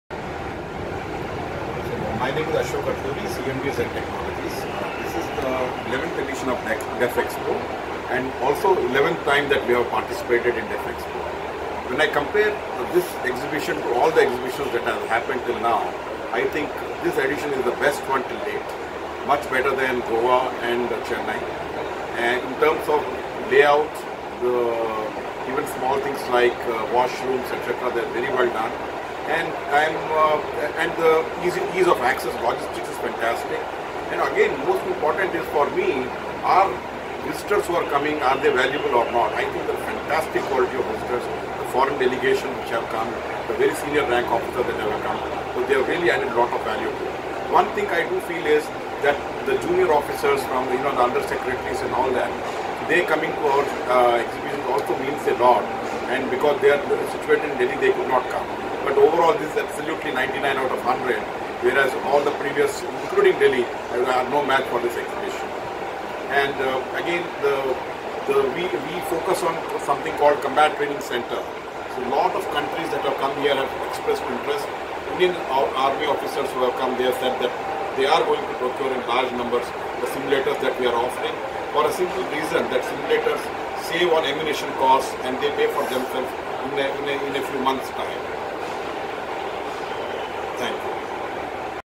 Ashok Atluri Chairman & Managing Director, Zen Technologies speaking exclusively to ADU Part 1
Ashok Atluri Chairman & Managing Director, Zen Technologies speaking exclusively to ADU at Defexpo 2020 in Lucknow Part 1